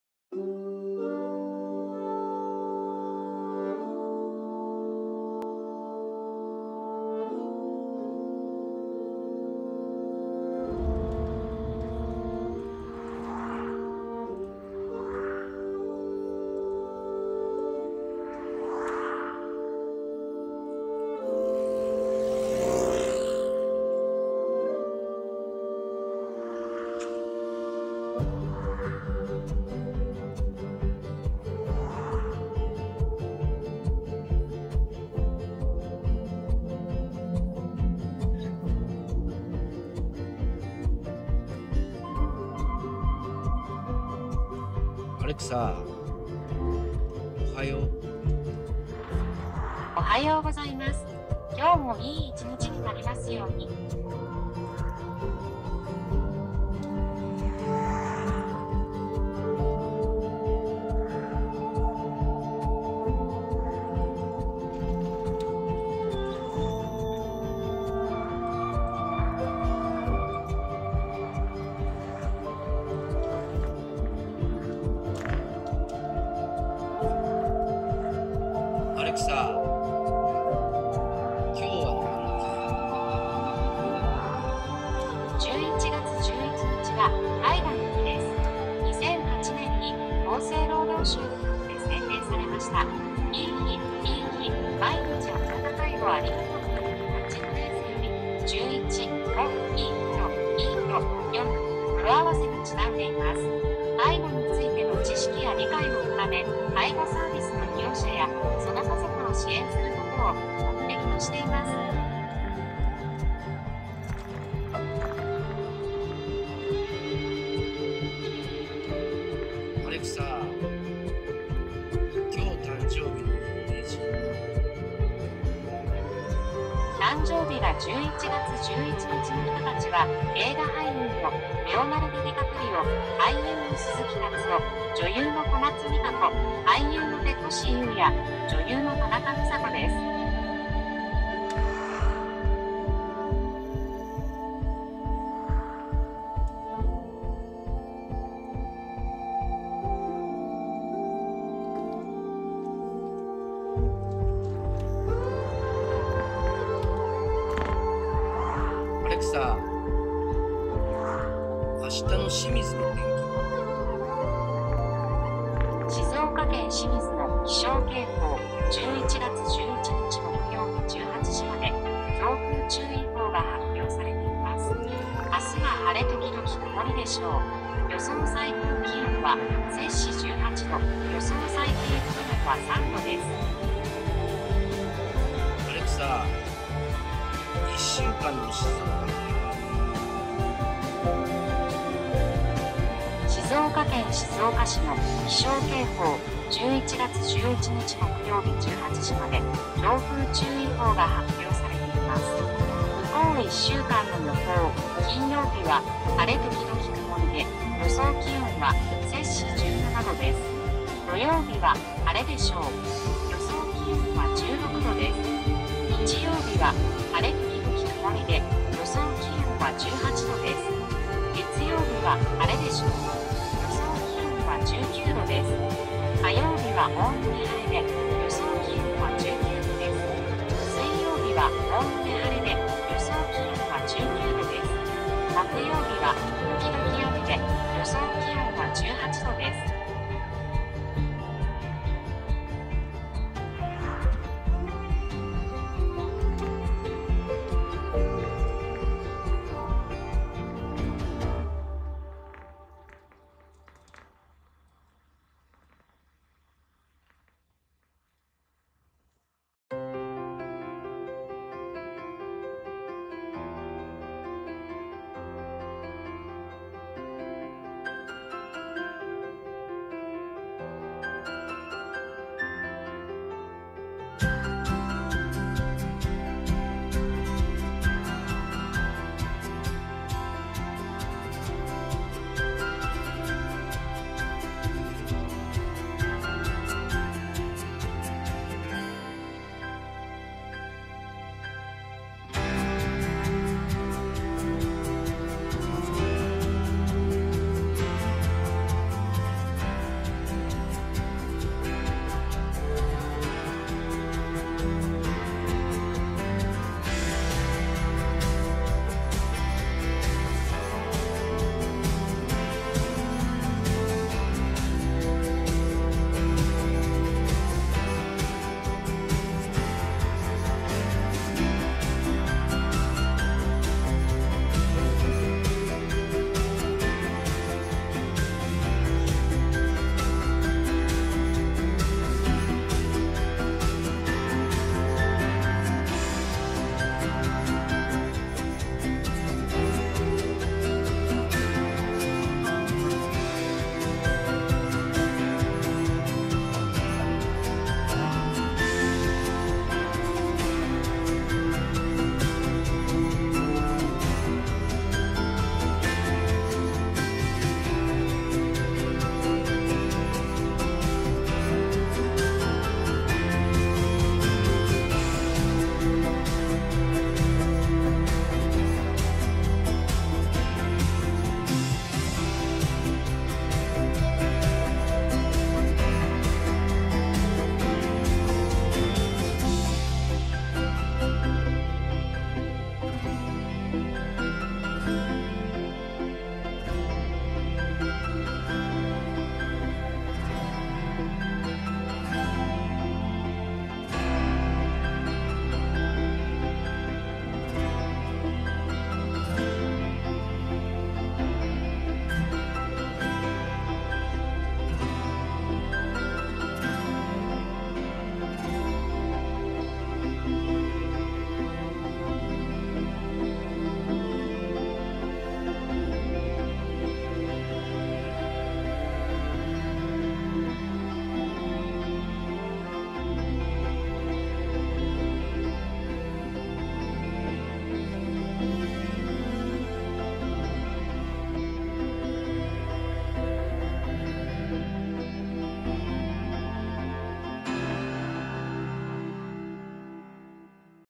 夕景の富士山 2021.11.11 三保街道と写真
2021年11月11日に撮影したちょこっと、ドライブ動画と写真です。